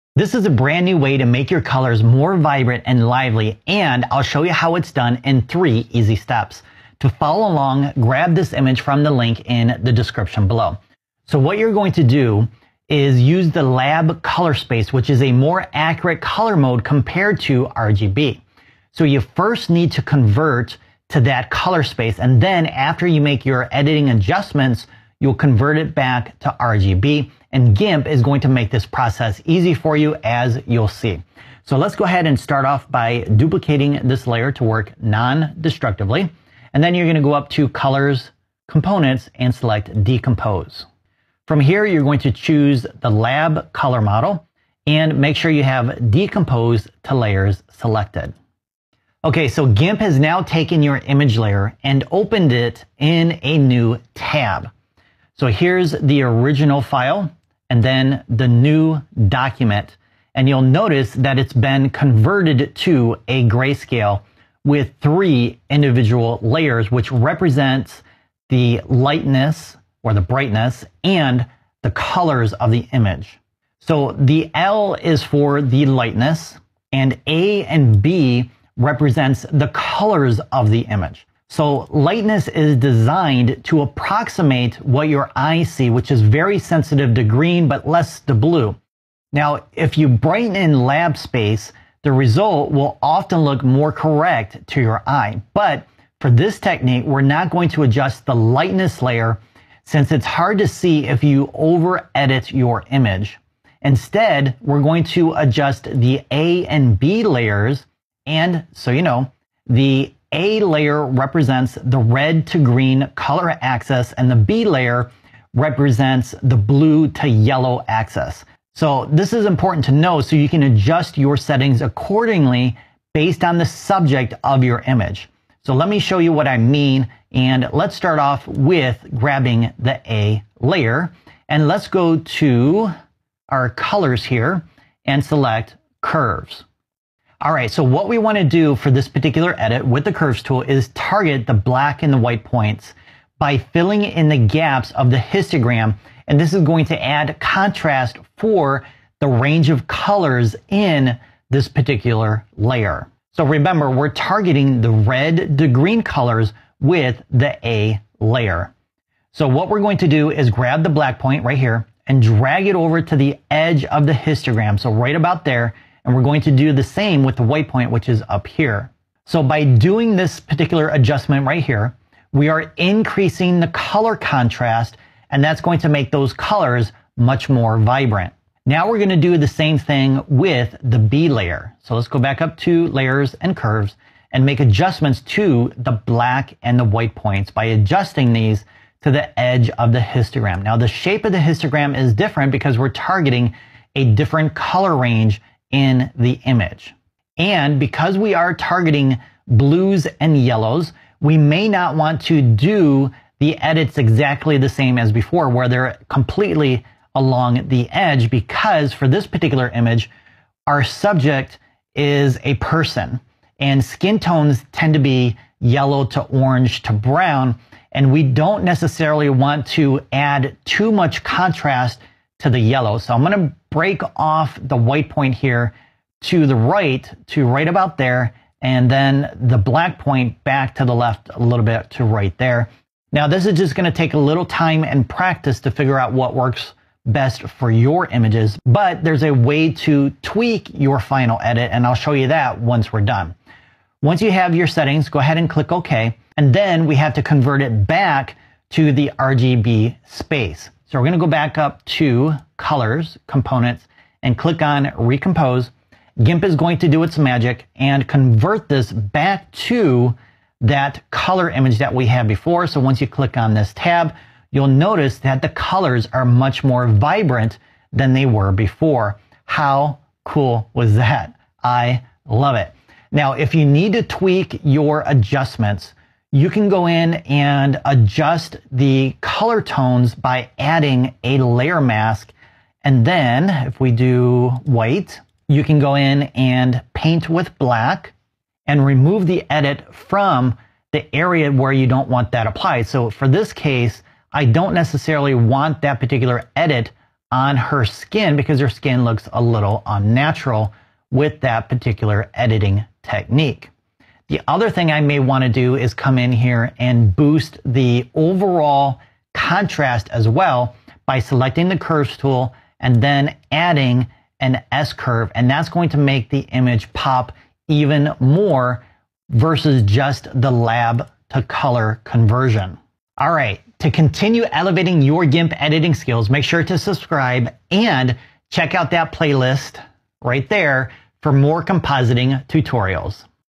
How To Boost Colors Of Your Photos with GIMP {Without the Levels or Curves Tool}
Discover a new GIMP editing technique to boost colors in your images.  Step-by-step instructions.

🅼🅰🆂🆃🅴🆁 🅶🅸🅼🅿 & 🆂🅰🆅🅴 🆄🅿 🆃🅾 80% 🅾🅵🅵!

📸 Worlds BEST Camera

BEST GIMP ALTERNATIVE...